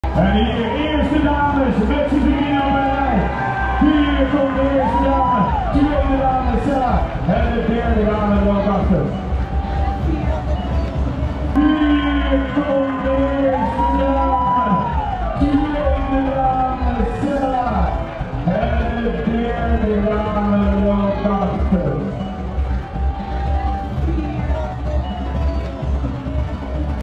Singelloop top 3 meisjes 2.5 kilometer Kids Run met slowmotions
Dit is de Singelloop top 3 meisjes 2.5 kilometer Kids Run met slowmotions

🛠 Tripod:
Manfrotto 504 HD
(amongst many)

⟶Stabilizer:
Ronin RS2

🖥  Edit:
Imac 27 inch scherm
MacBook Pro

📞 Mobile phone:
Iphone Pro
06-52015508